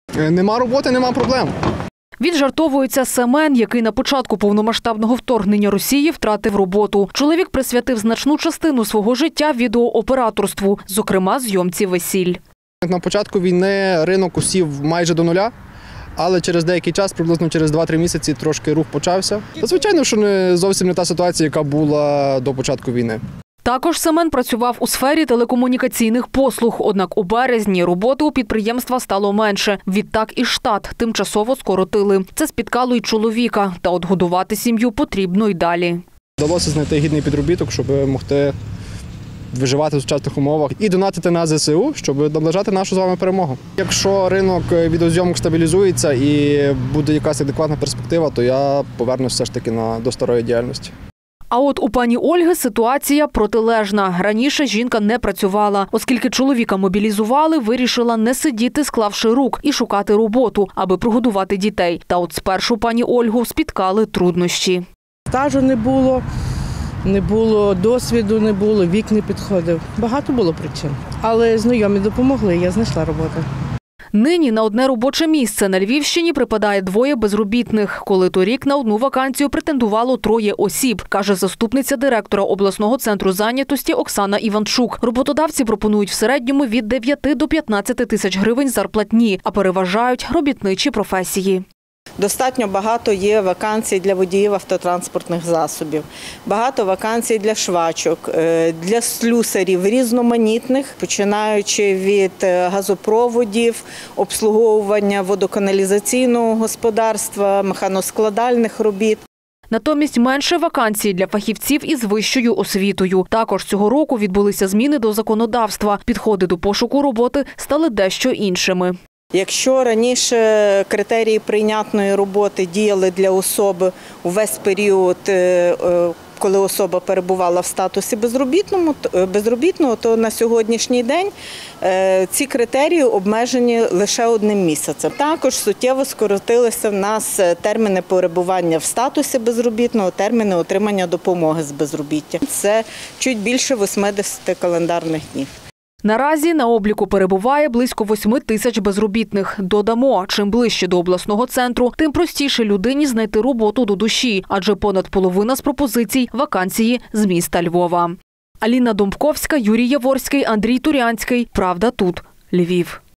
Робота під час війни: яка ситуація з працевлаштуванням на Львівщині?
На обліку в обласному центрі зайнятості перебуває близько 8 тис безробітних. На 1 робоче місце претендують 2 осіб.